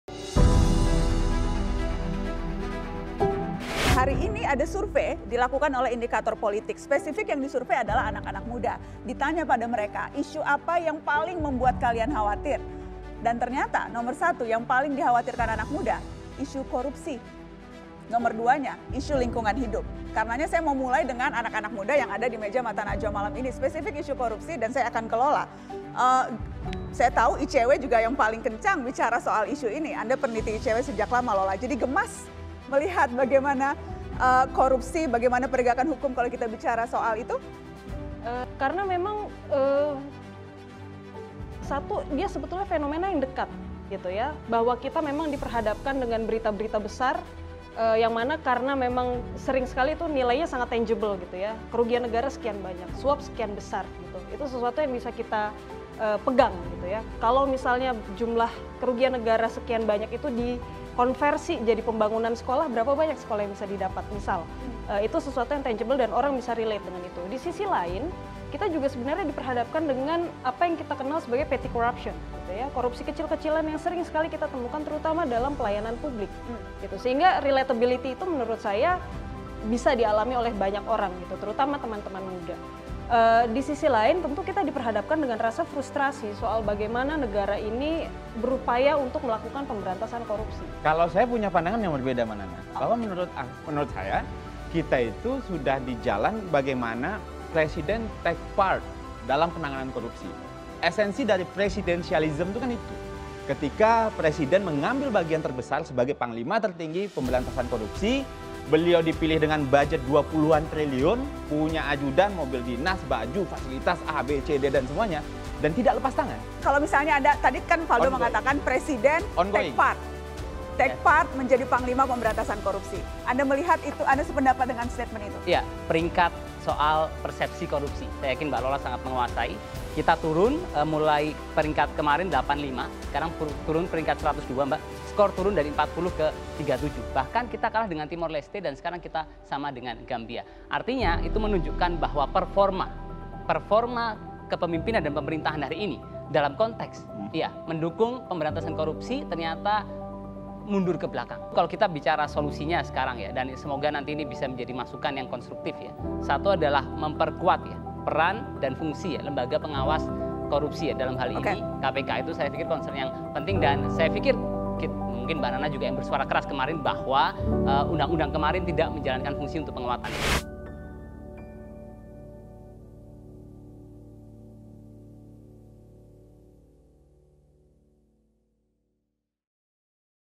Anak Muda Paling Khawatir Isu Korupsi. ICW: "Kami Frustasi" | Narasi Newsroom
Survei Indikator Politik bulan September kemarin bilang kalau korupsi jadi isu yang paling dikhawatirkan anak muda.⁣⁣

Kata Lalola Ester Kaban, peneliti ICW, hal ini bisa terjadi soalnya masalah korupsi ini tangible dan dekat dengan kehidupan kita. Miris, sih, sebenarnya, yaa?⁣⁣

Kalo kata Stafsus Mensesneg, Faldo Maldini, kita sekarang udah di jalan kalau Presiden ambil bagian jadi panglima tertinggi pemberantasan korupsi.⁣⁣

Kamu setuju enggak?⁣⁣

(Narasi)